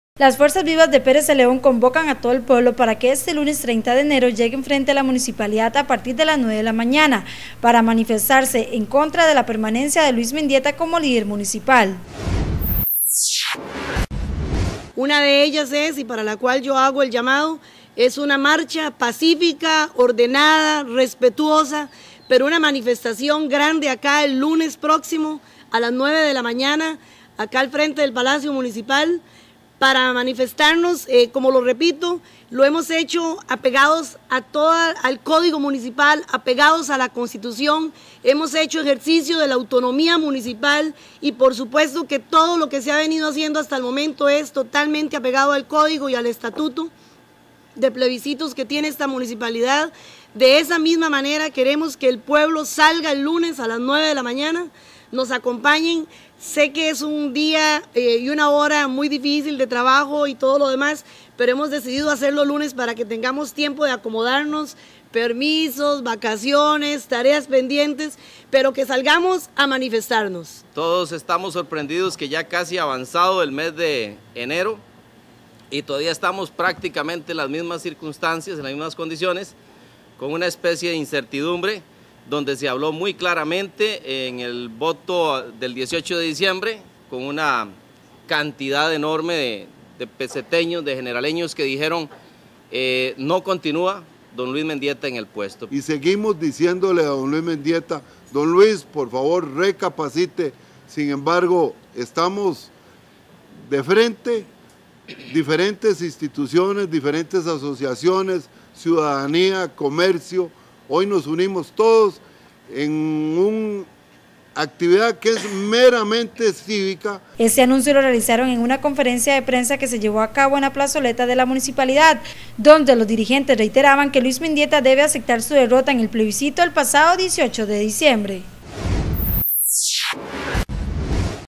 Convocan a manifestación este lunes contra alcalde
Las fuerzas vivas de Pérez Zeledón convocan a todo el pueblo para que este lunes 30 de enero lleguen frente a la municipalidad a partir de las nueve de la mañana, para manifestarse en contra de la permanencia de Luis Mendieta como líder municipal.